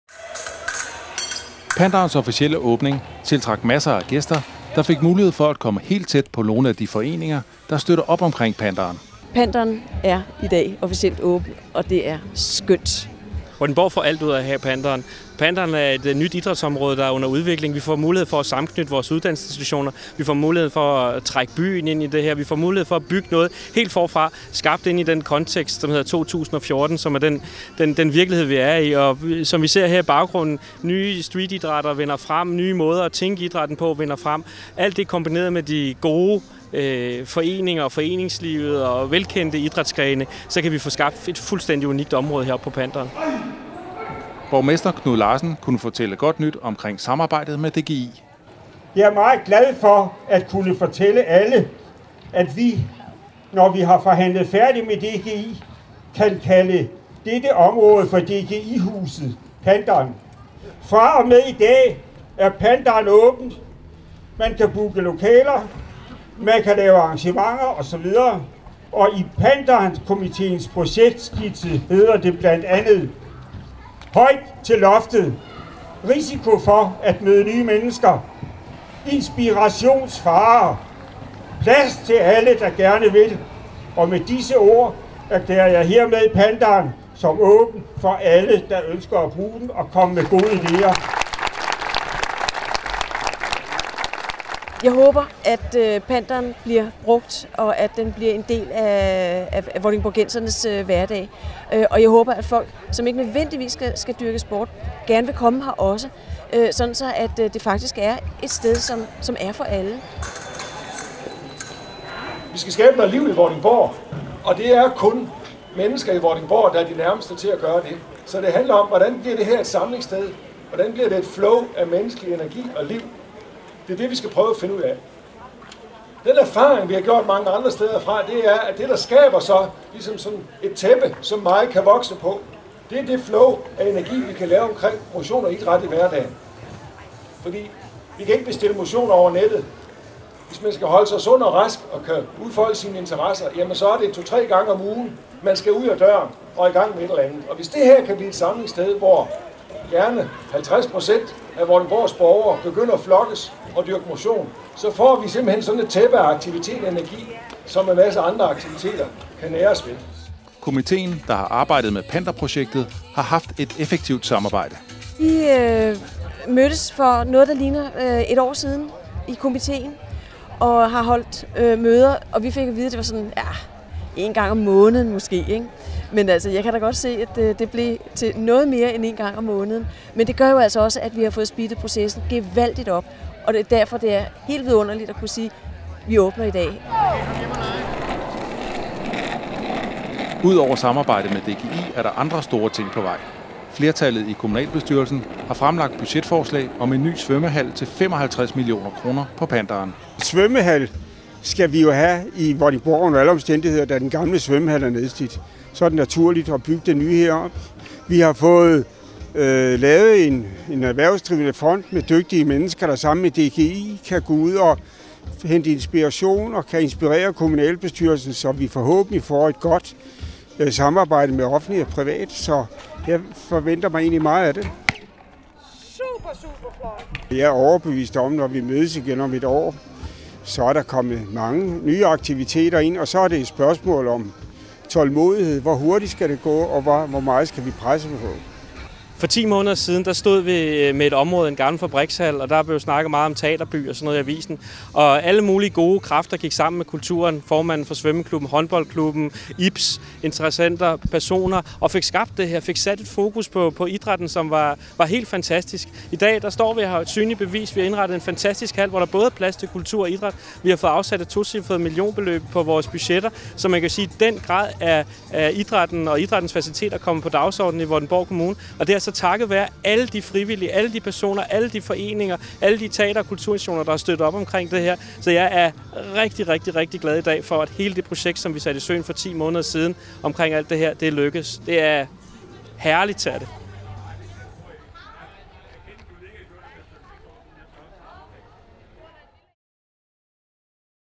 PANTEREN i Vordingborg - Officiel åbning
Lørdag d. 20. september 2014 blev PANTEREN i Vordingborg officielt åbnet. Det betyder starten på en spændende udvikling om et helt nyt område i Vordingborg med masser af aktiviteter.